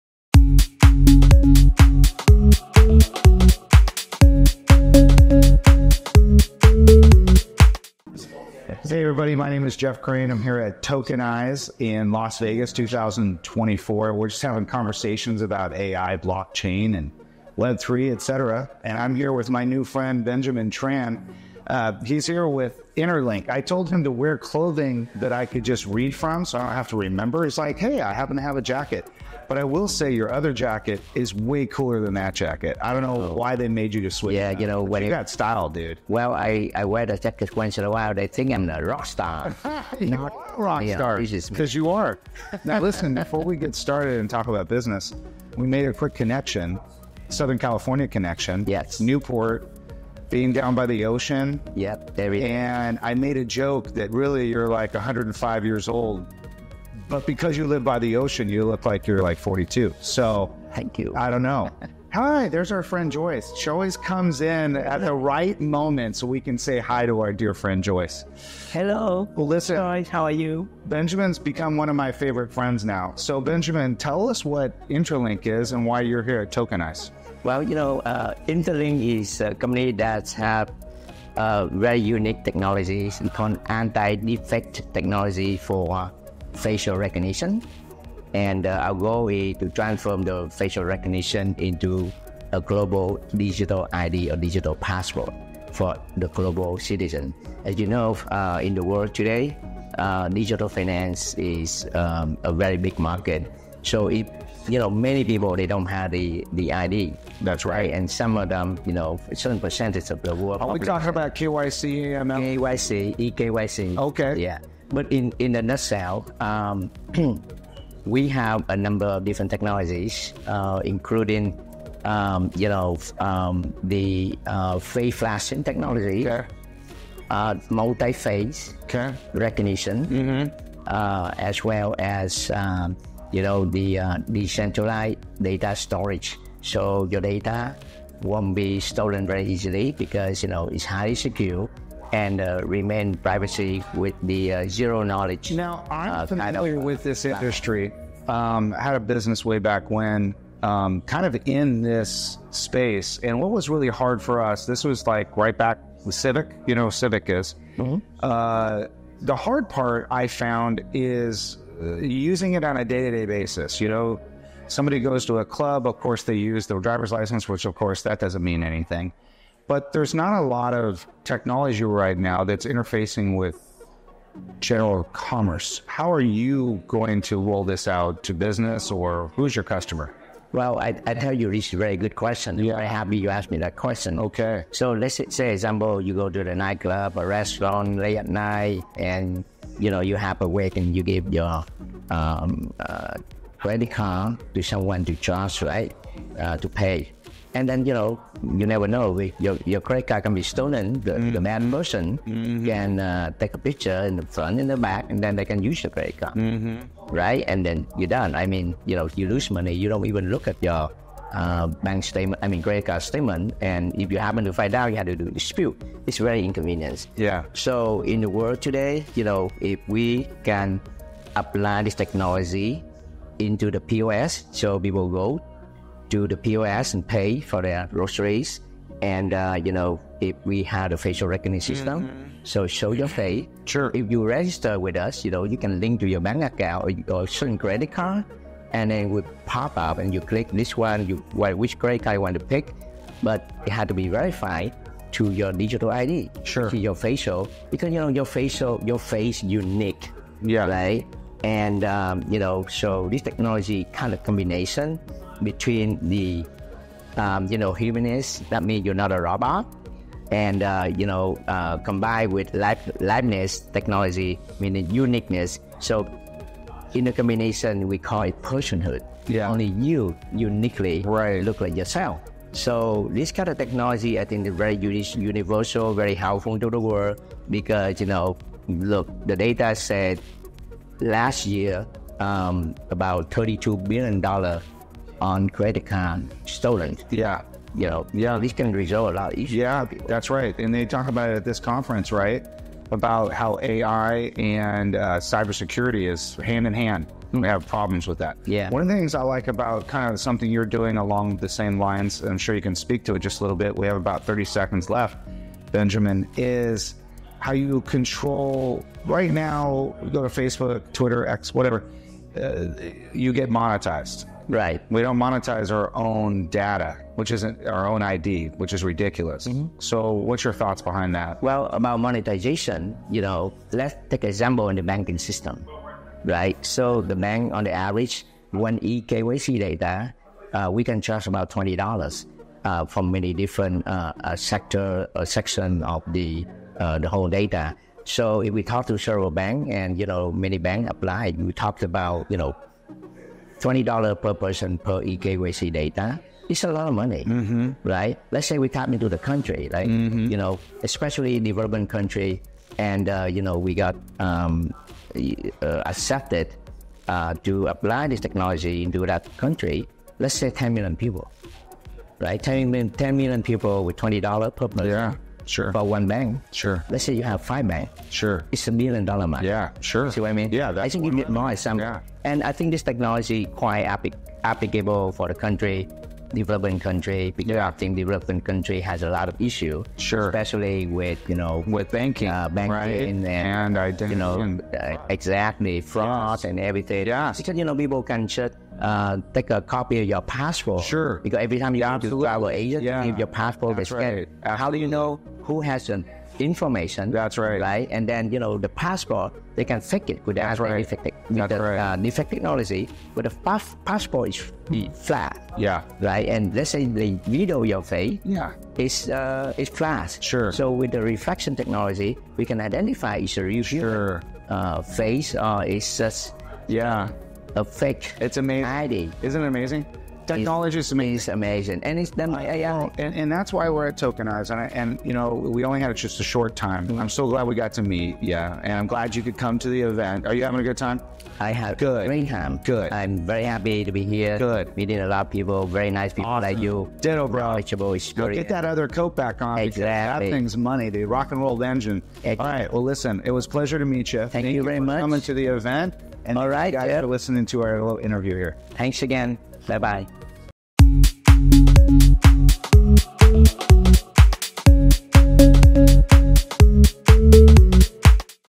DNA Presents Interlink Labs @ Tokenize! Las Vegas 2024
Dive into an insightful interview with Benjamin Tran of Interlink Labs, where we explore the cutting-edge advancements transforming the tech landscape. Benjamin shares Interlink Labs' innovative approach to creating seamless solutions that bridge technology and everyday life. Gain exclusive insights into the company's vision, groundbreaking projects, and Benjamin’s thoughts on the future of the industry.